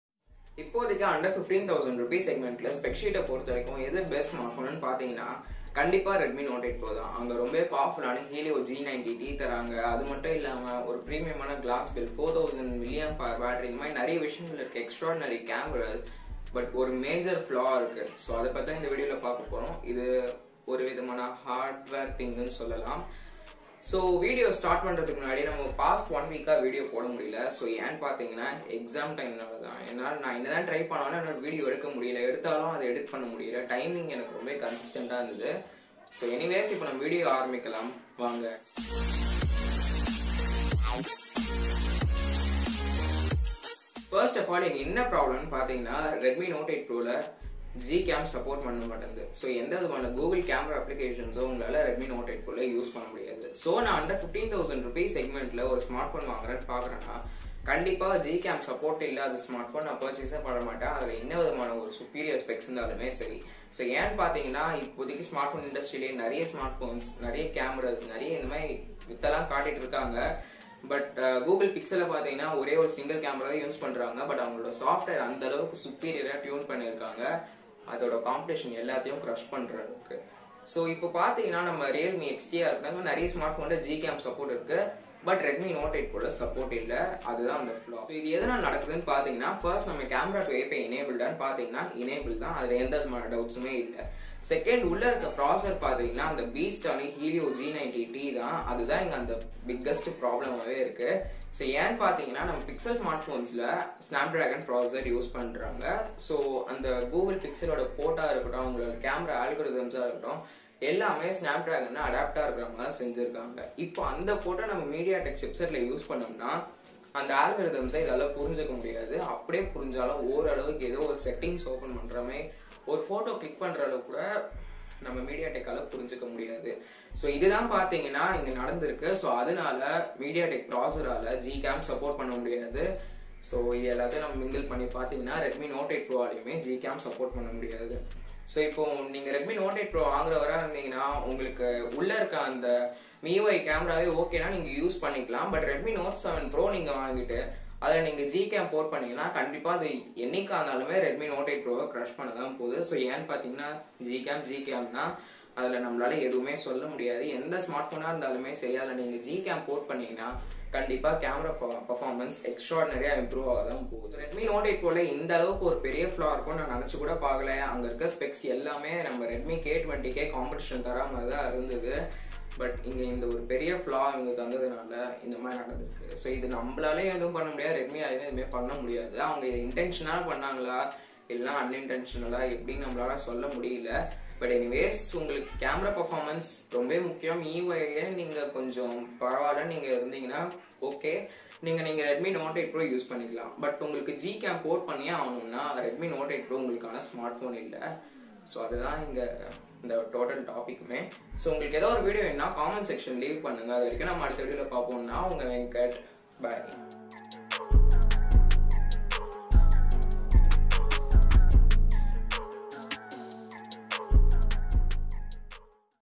Redmi Note 8 Pro - No GCAM support| Tech Evo Tamil
Redmi note series thaan yeppavume one of the best budget phones ahh irundhu iruku, ippo andha line up la note 8 pro Vandhuruku yella specs umm ehh super ahh thaan iruku but ORU MAJOR FLAW IRUKU adhu innanu paathinga na Gcam support pannadhu, idhu yen support aaguthu dhaan indha video la cover pannirukan .